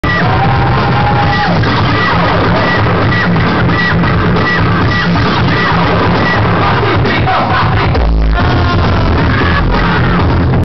Miss Kittin @ dkd, Shanghai [04-16-05] pt.6
miss kittin live in club dkd, shanghai, china.